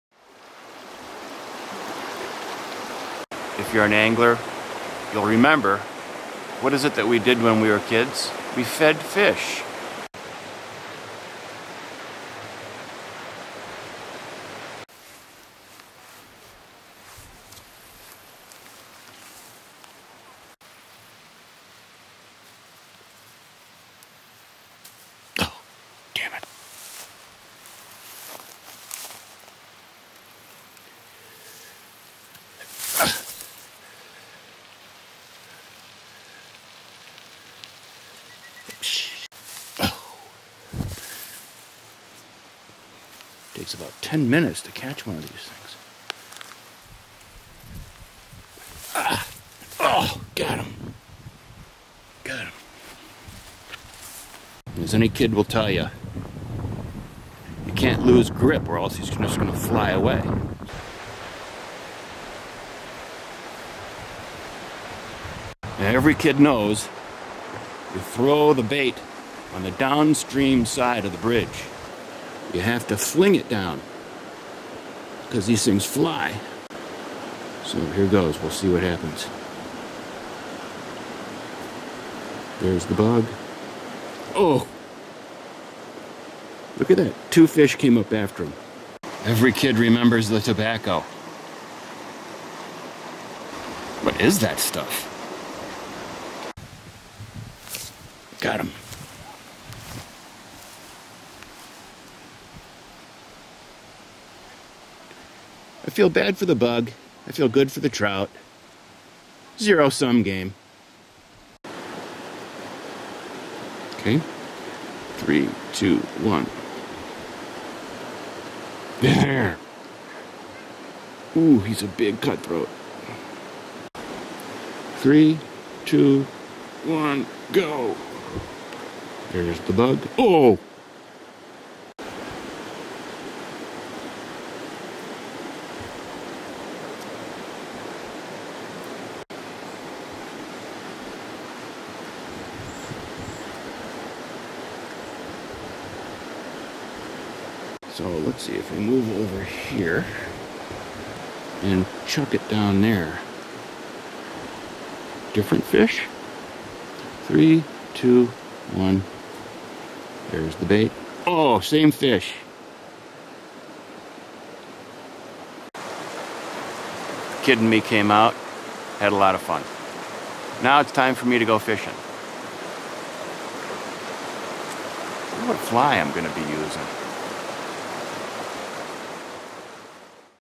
The Hopper Dropper
A kid, some trout; a bridge and grasshoppers.  While I'd rather be fishing, feeding the fish is fun too.